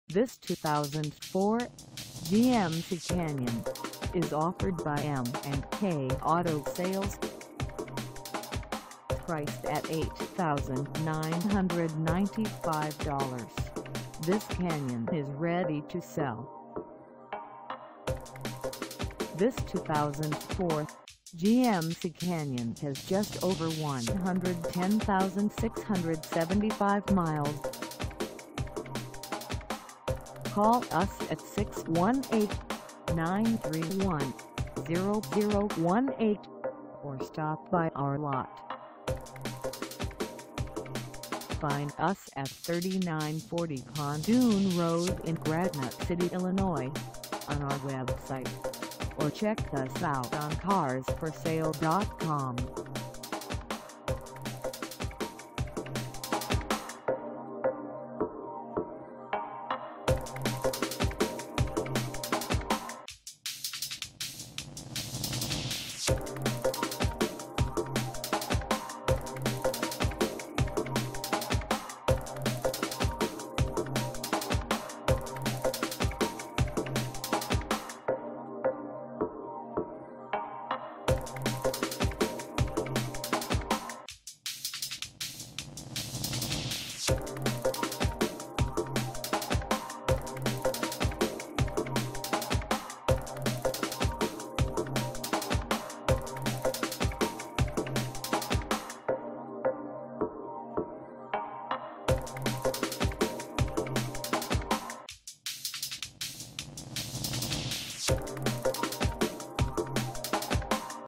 2004 GMC Canyon SLE Z85 Ext. Cab 2WD - for sale in Granite C
M and K Auto Sales
3940 Pontoon Rd
in Granite City, IL 62040

Come test drive this 2004 GMC Canyon SLE Z85 Ext. Cab 2WD for sale in Granite City, IL.

VERY CLEAN AND HARD TO FIND-WON'T LAST LONG